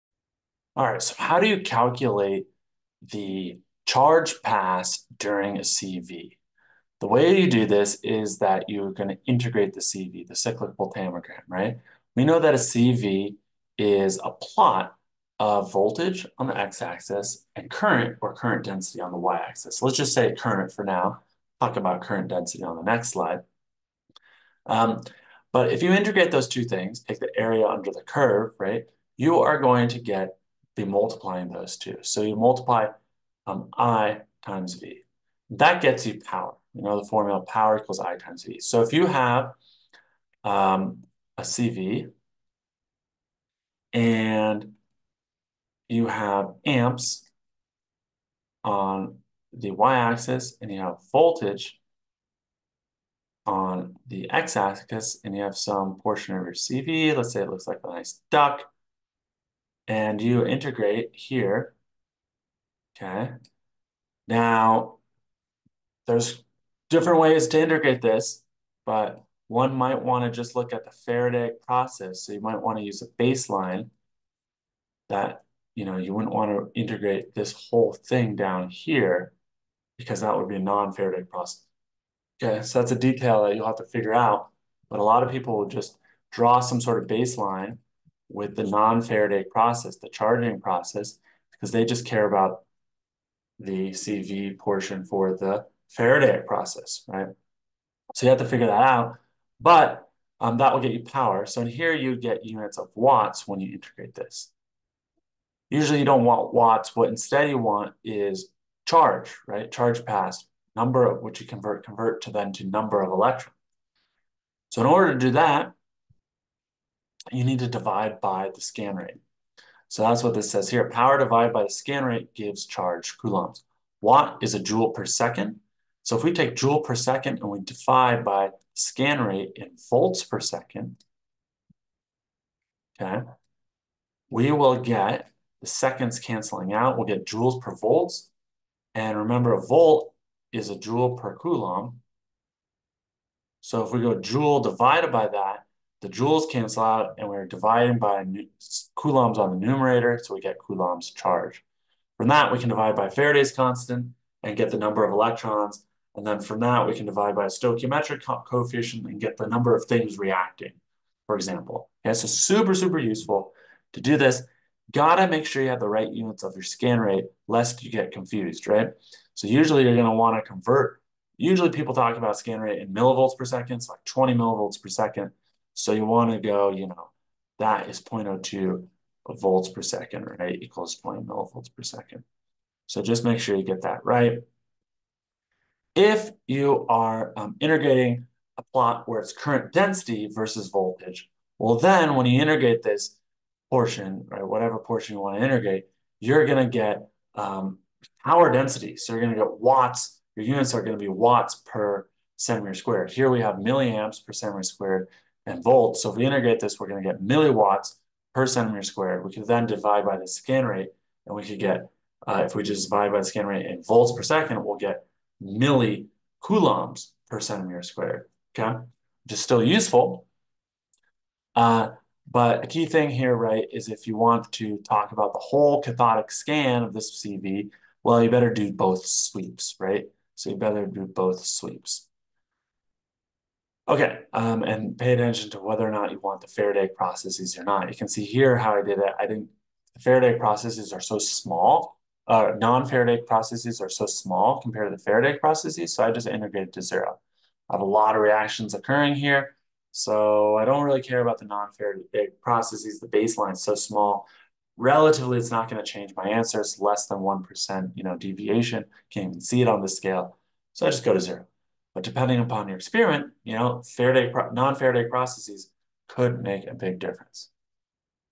Calculating Charge Passed During Cyclic Voltammetry
Calculating the charge passed during a cyclic voltammogram, integrate and then divide by the scan rate. Watch your units! Also, make note of non-Faradaic and Faradaic processes.